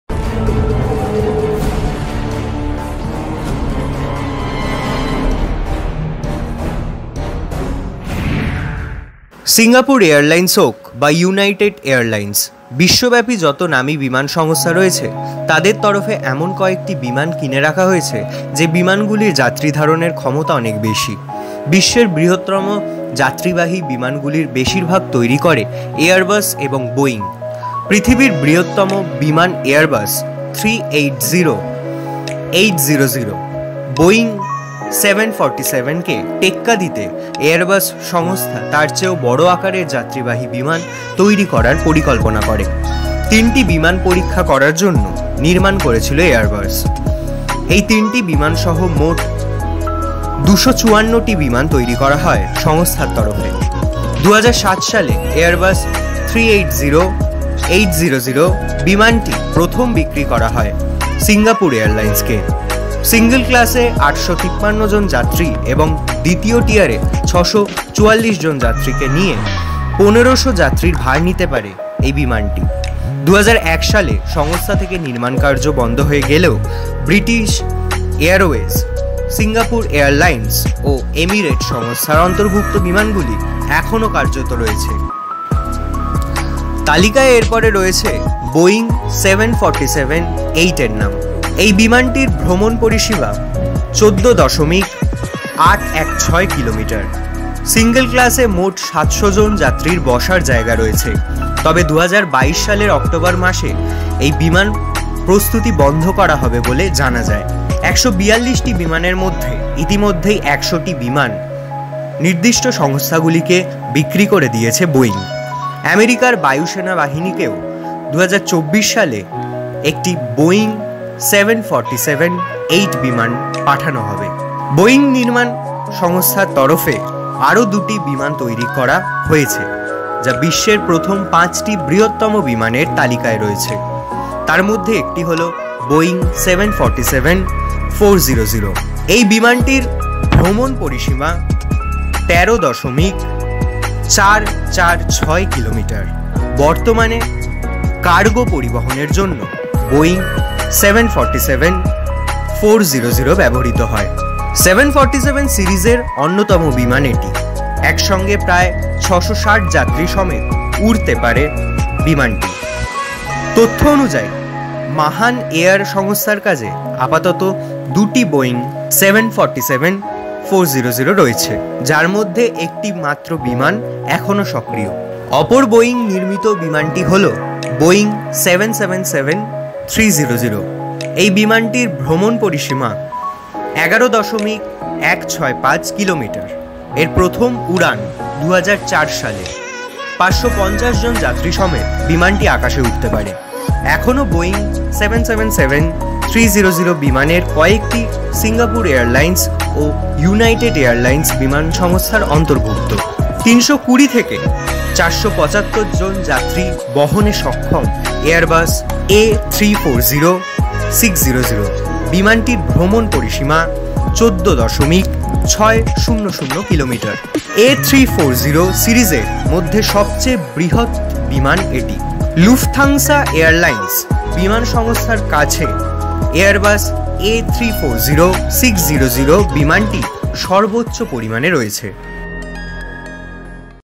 Largest Passenger Aircraft: বিশ্বের বৃহত্তম যাত্রিবাহী বিমান, একসঙ্গে বহন করতে পারে ১৫০০ যাত্রী!
Largest Passenger Aircraft : ‘সিঙ্গাপুর এয়ারলাইনস’ হোক বা ‘ইউনাইটেড এয়ারলাইনস’— বিশ্বব্যাপী যত নামী বিমানসংস্থা রয়েছে, তাদের তরফে এমন কয়েকটি বিমান কিনে রাখা হয়েছে যে বিমানগুলির যাত্রীধারণের ক্ষমতা অনেক বেশি। আসুন দেখে নিই এমন এক বিমান যে বহন করতে পারে দেড় হাজারেরও বেশি যাত্রী।